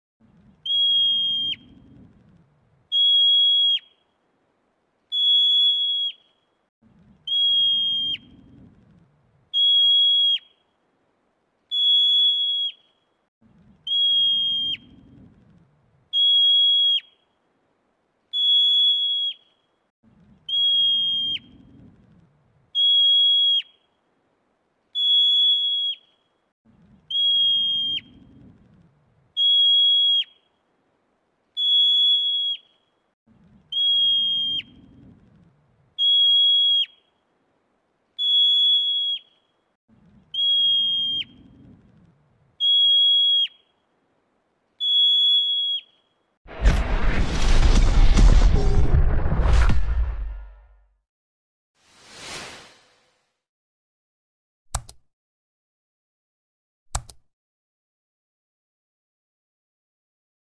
Marmot sound effect|Sound the Alarm
Hello friends Welcome to the amazing world of animal sounds.Marmots are large ground squirrels in the genus Marmota, with 15 species living in Asia, Europe, and North America. These herbivores are active during the summer, when they can often be found in groups, but are not seen during the winter, when they hibernate underground. They are the heaviest members of the squirrel family.
Thanks
Bless up!😎